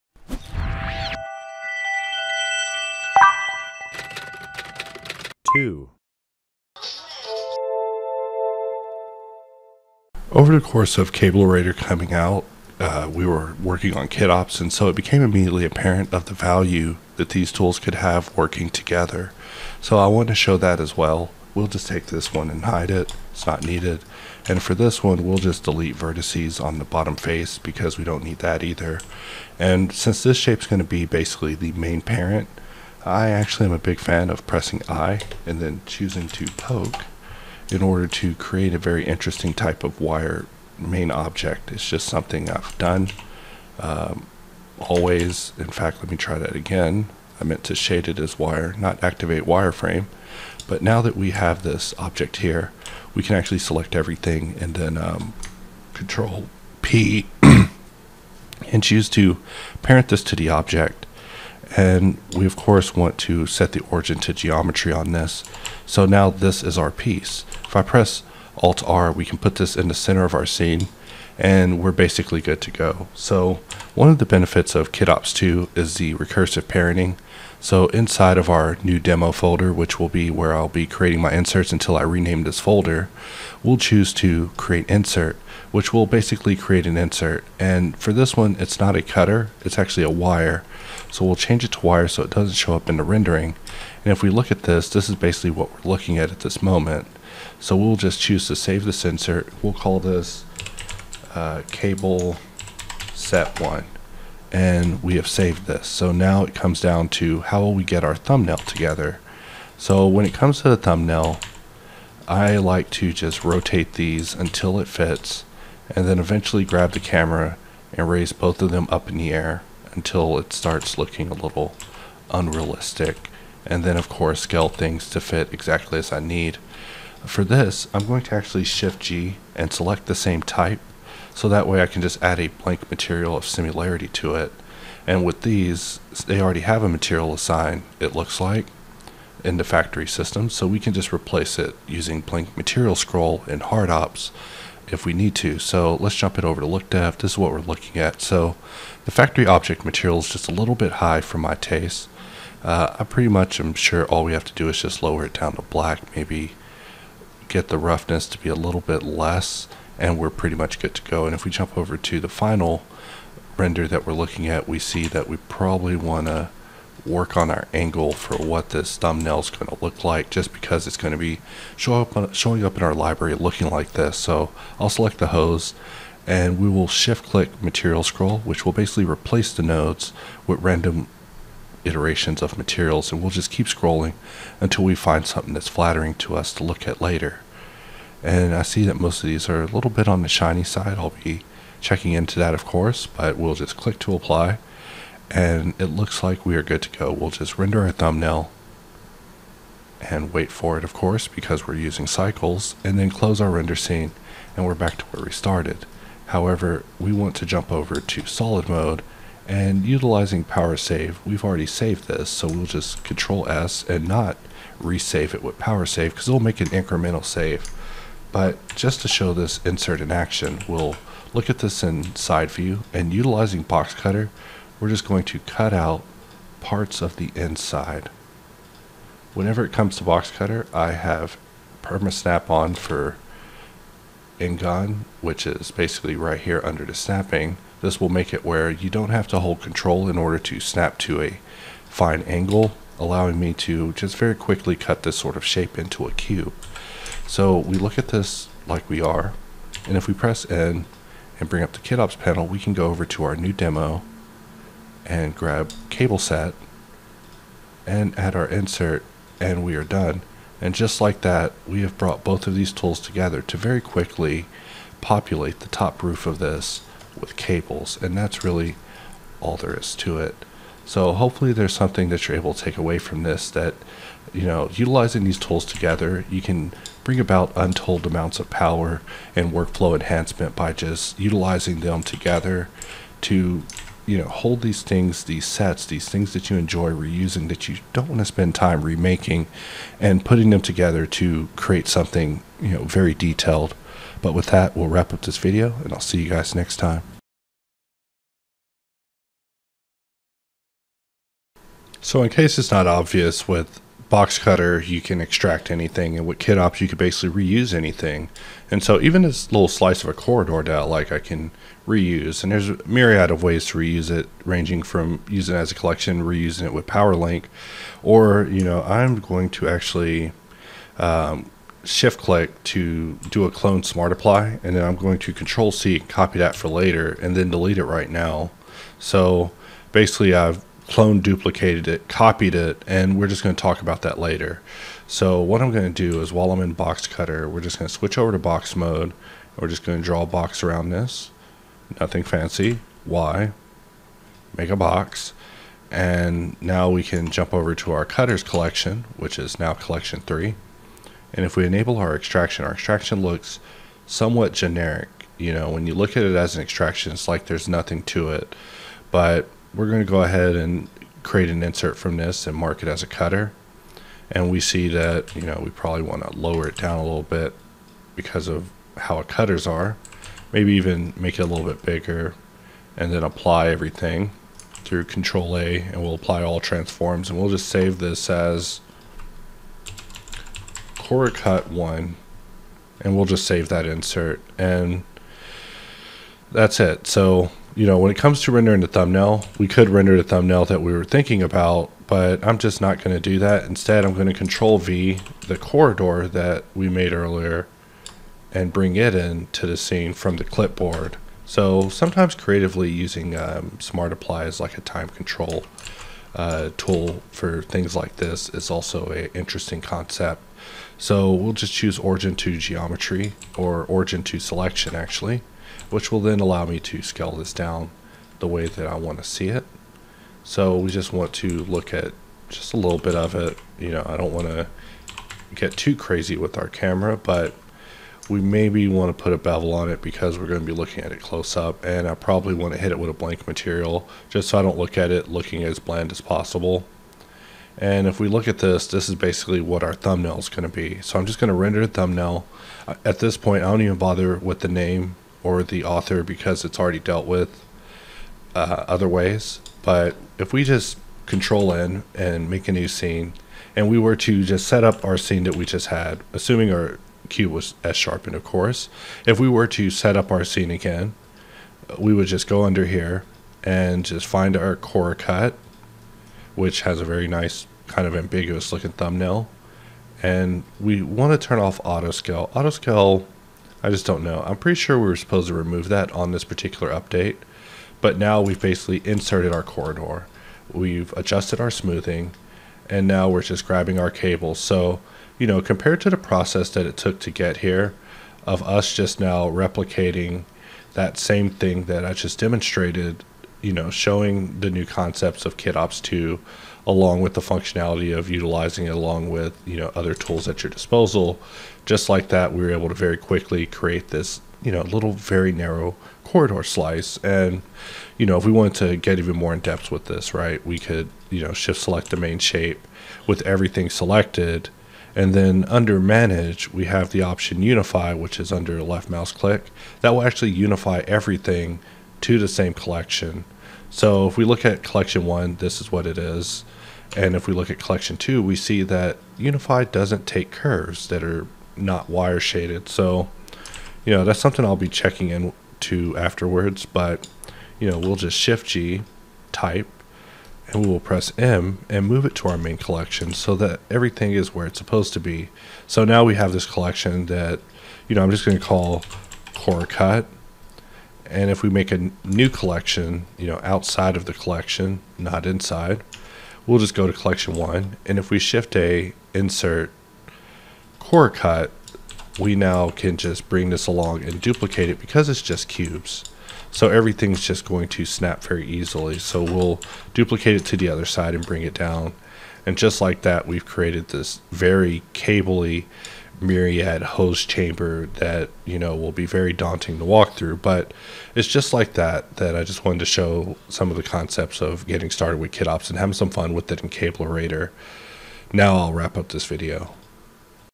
kitops2 w/ Cablerator
In this video I aim to show its value when used w/ kitops2. While Cablerator has made adding connectors to multiple objects easier, Kitops2 is able to reuse almost anything desired at this point in time. Let's dive more in depth in this video.

Try... anywhere. Just try.